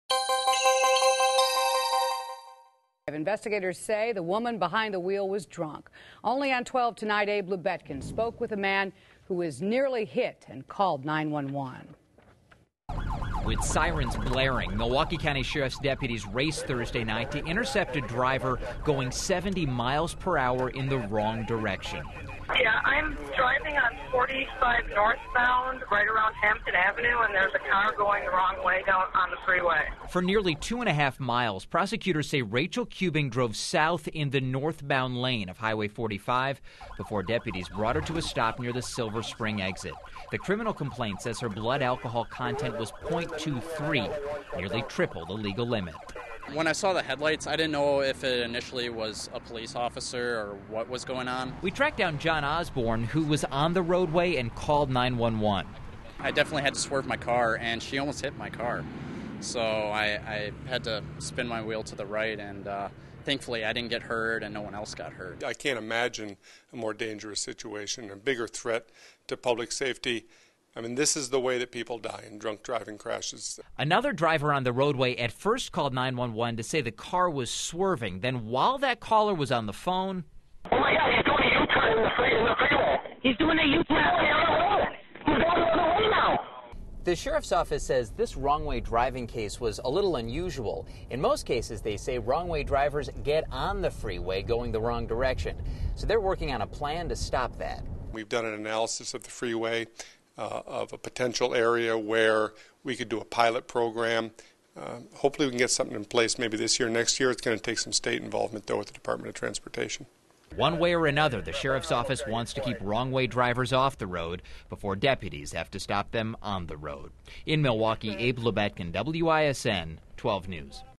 Wrong Way Diver Caught On Camera
Department of Transportation cameras and Sheriffs Department squad dash cameras catch wrong way driver on US 45 in Milwaukee county.